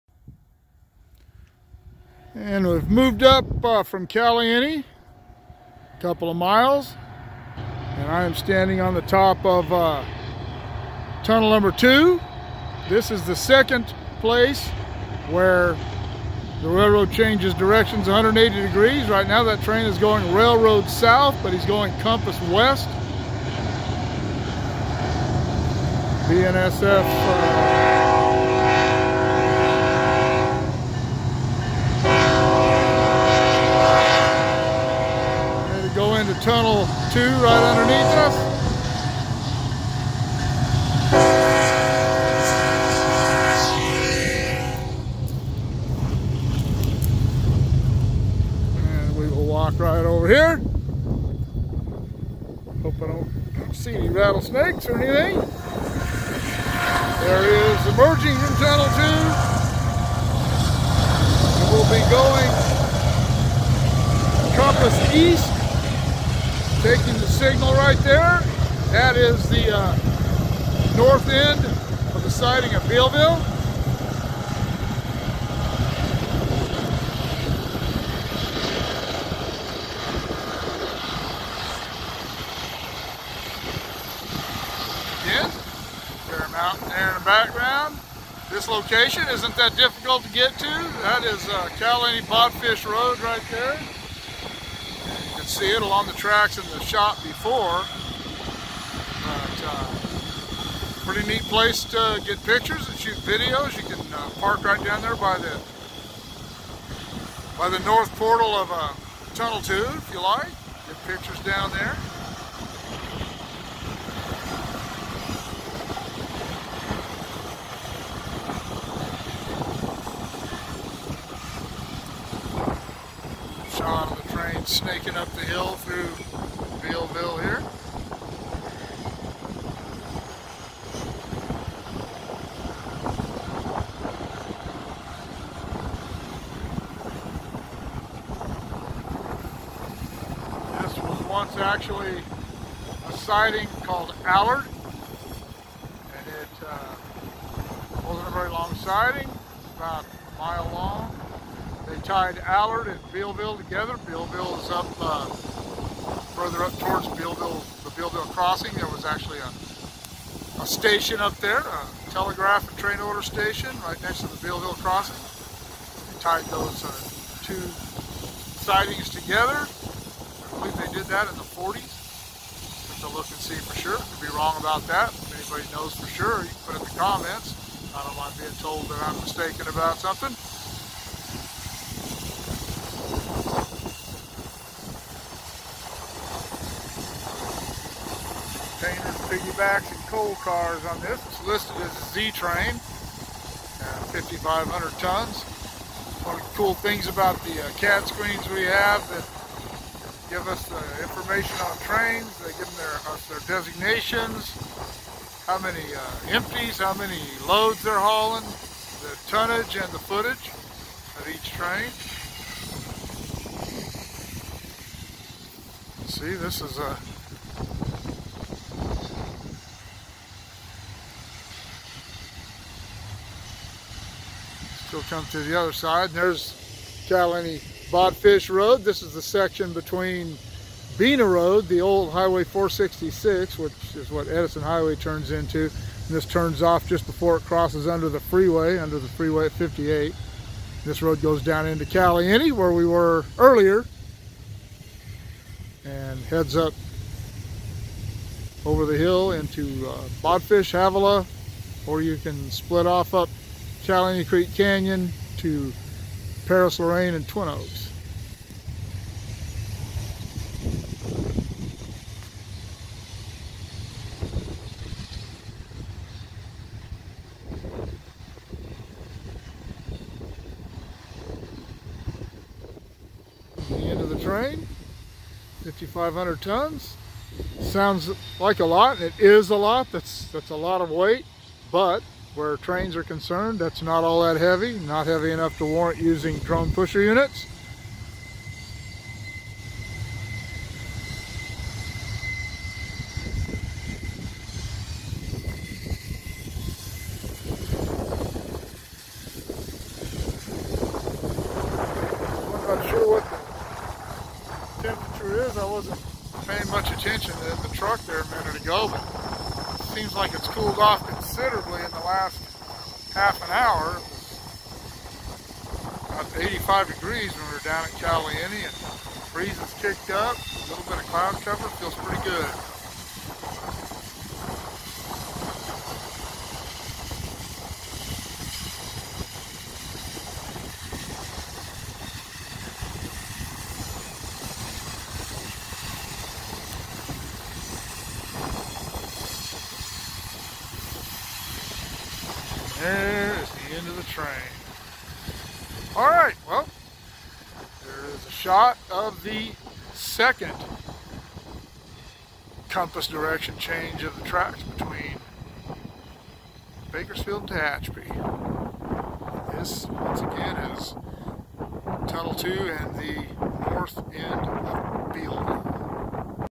Railroading on the Tehachapi: BNSF eastbound at Tunnel 2
This is the second location on the mountain where the tracks make a 180 degree direction change.
Tunnel 2 is in the middle of the initial curve followed by the north end of Bealville at MP 338. The direction change doesn't fully occur for about another quarter mile.
It's cool to be able to stand over the tunnel and watch the train enter and exit while only having to walk a few feet to see it.
So join me for this installment of the crazy direction changes of the Tehachapi!
Follow these links to the other horseshoe curves.
Tunnel 2 GPS  35d 17' 09"N  118d 28' 39"W